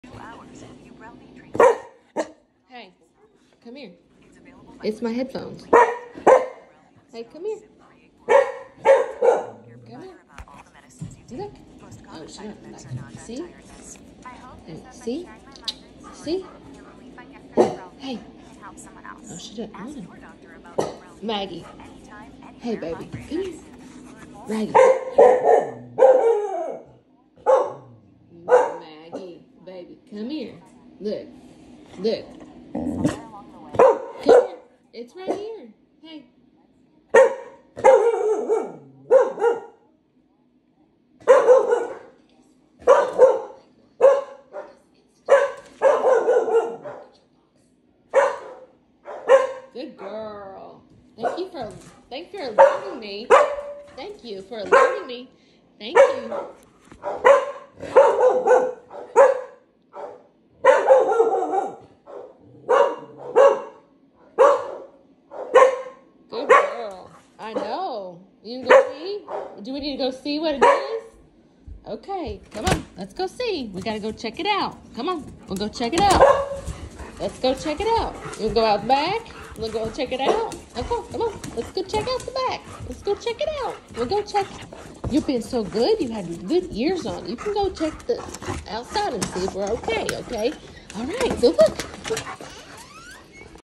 Dog is alert and ready to guard. That’s my aussie!
It was my headphones. But I still let them out to check.